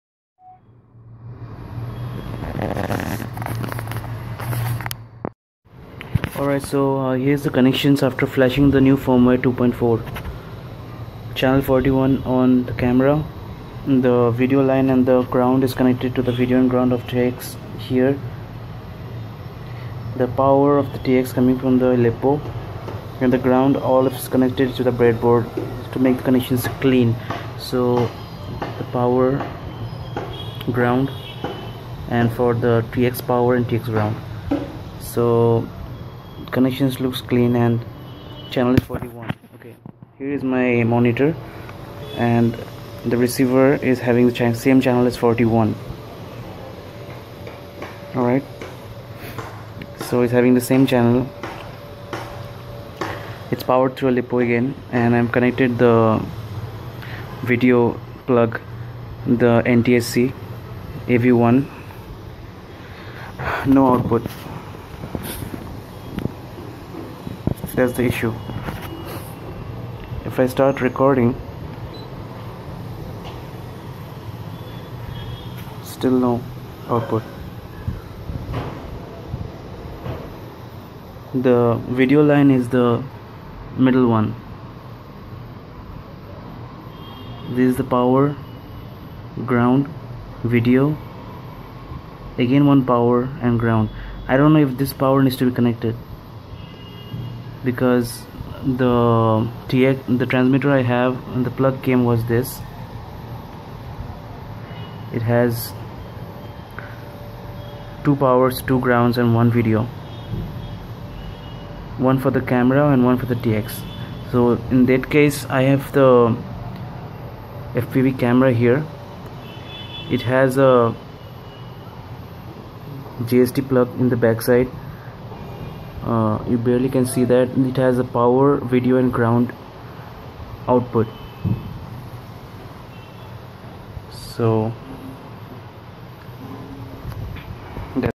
No TV out after flashing new firmware for Runcam v2.4!!!!
There is no TV out coming from the fpv monitor aftr I flashed the new firmware v2.4